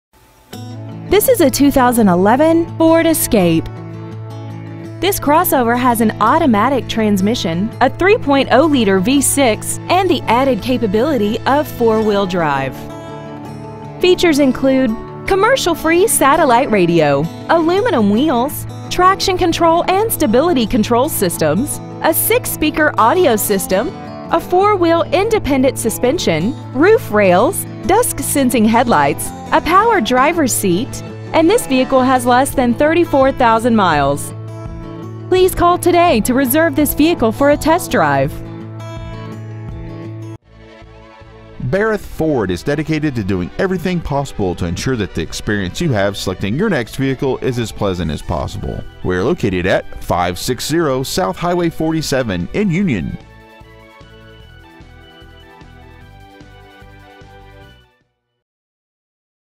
Preowned 2011 FORD ESCAPE Union MO
We have been honored to serve the Union MO area , we promise that your experience at our dealership will exceed your expectations !

 Year : 2011
 Make : FORD
 Model : ESCAPE
 Engine : 3.0L V6 24V MPFI DOHC FLEXIBLE FUEL
 Trans . : 6-SPEED AUTOMATIC
 Exterior : STERLING GRAY METALLIC
 Miles : 33,677
 Interior : CHARCOAL BLACK
 Stock : R1951